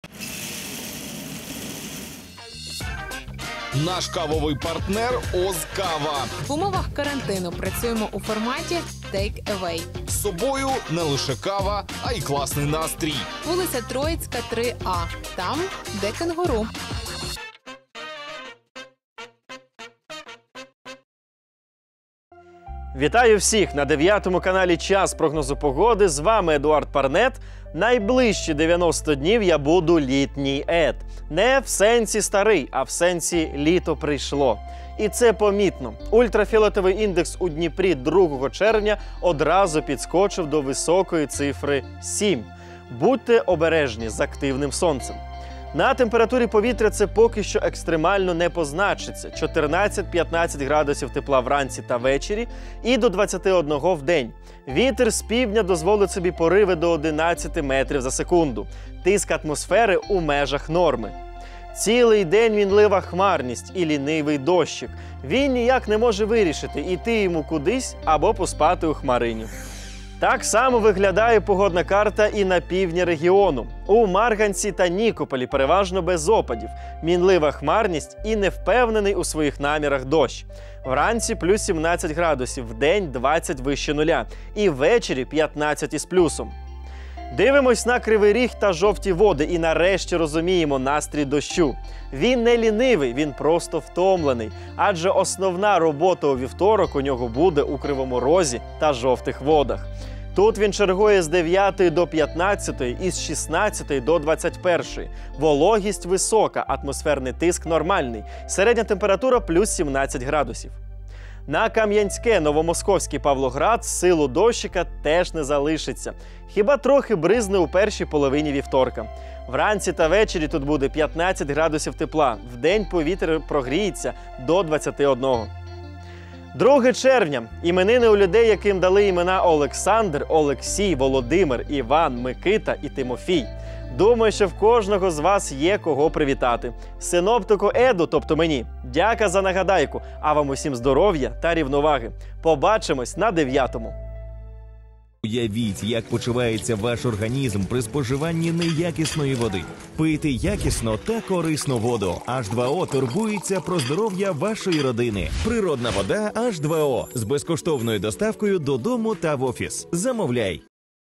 Літній Ед. Прогноз погоди на 2 червня, вівторок. Дніпро і область
Вітаю всіх, на 9 каналі час прогнозу погоди, з вами Едуард Парнет. Найближчі 90 днів я буду Літній Ед. Не в сенсі “старий”, а в сенсі — літо прийшло!!!
І це помітно — ультрафіолетовий індекс у Дніпрі 2 червня одразу підскОчив до високої цифри 7- будьте обережні з активним сонцем. На температурі повітря це поки що  екстремально не познАчиться — 14-15 градусів тепла вдень і ввечері і до 21 вдень. Вітер з півдня дозволить собі пориви до 11 метрів за секунду. Тиск атмосфери у межах норми. Цілий день мінлива хмарність і лінивий дощик. Він ніяк не може вирішити — іти йому кудись або поспати у хмарИні.
Так само виглядає погодна карта і на півдін регіону. У Марганці та Нікополі переважно без опадів, мінлива хмарність і невпевний у своїх намірах дощ. Вранці +17 градусів, вдень — 20 вище нуля і ввечері — 15 із плюсом.
Дивимось на Кривий Ріг та Жовті Води і нарешті розуміємо настрій дощу — він не лінивий, він просто втомлений, адже основна робота у вівторок у нього буде у Кривому Розі та Жовтих Водах. Тут він чергує з 9-ї до 15 і з 16 до 21-ї. Вологість висока, атмосферний тиск нормальний, середня температура +17 градусів.

На Кам’янське, Новомосковськ і Павлоград сил у дощика теж не залишиться — хіба трохи бризне у першій половині вівторка. Вранці та ввечері тут буде 15 градусів тепла, вдень повітря прогріється до 21-го.
Друге червня — іменини у людей, яким дали імена Олександр, Олексій, Володимир, Іван, Микита і Тимофій. Думаю, що в кожного з вас є, кого привітати. Синоптику Еду, тобто мені — дяка за нагадайку, а вам усім — здоров'я та рівноваги. Побачимось на 9-му.